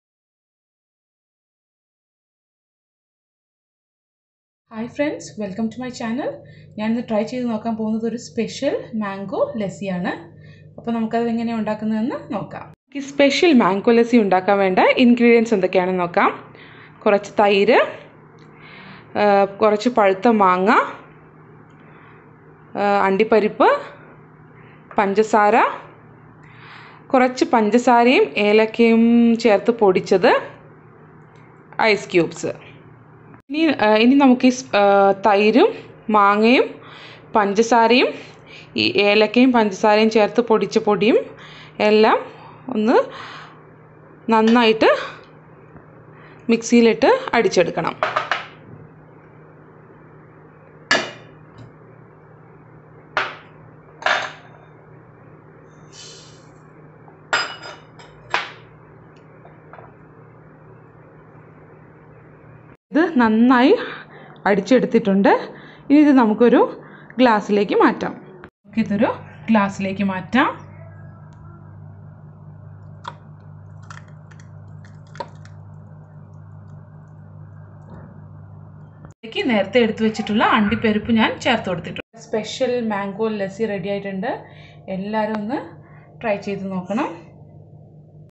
Special Mango Lassi || Summer cool || Easy recipe Kerala style
This is an easy drink we can make during summer season..Just 2 min recipe..

Ingredients
Mango
Curd
Sugar
Elachi powder
Ice cubes
Cashew